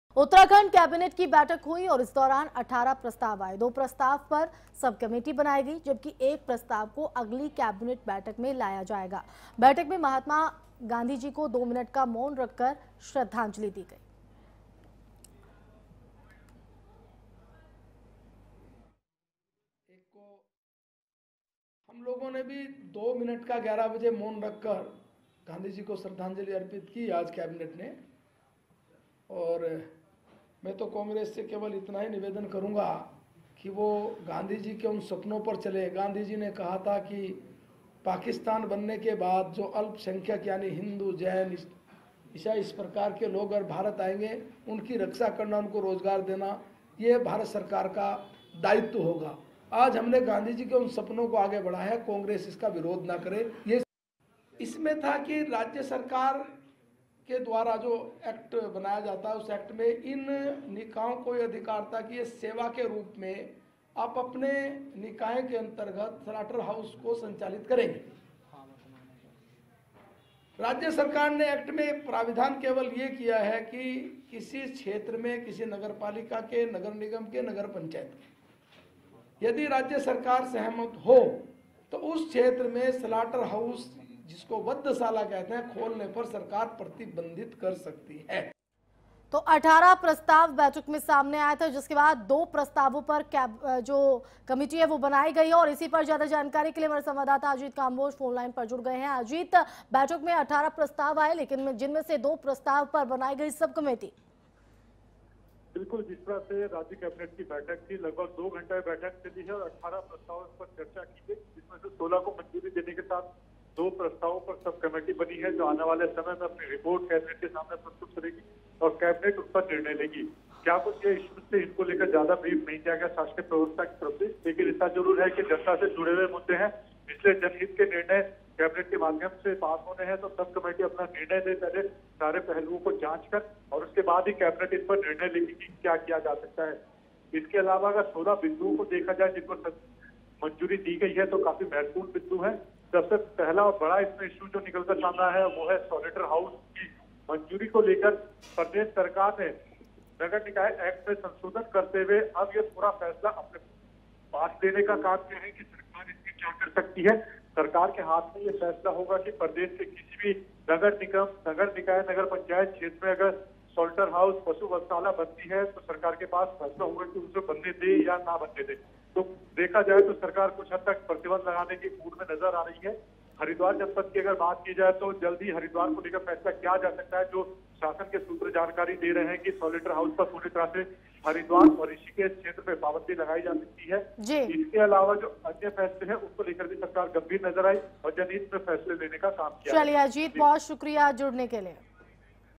उत्तराखंड कैबिनेट बैठक: बैठक में आए 18 प्रस्ताव, दो प्रस्ताव पर सब कमेटी बनाई गई R9TV
उत्तराखंड कैबिनेट की बैठक हुई .. इस दौरान 18 प्रस्ताव आए .. दो प्रस्ताव पर सब कमेटी बनाई गई है, जबकि एक प्रस्ताव को अगली कैबिनेट बैठक में लाया जाएगा.. बैठक में महात्मा गांधीजी को दो मिनट का मौन रखकर श्रद्धांजलि दी गई...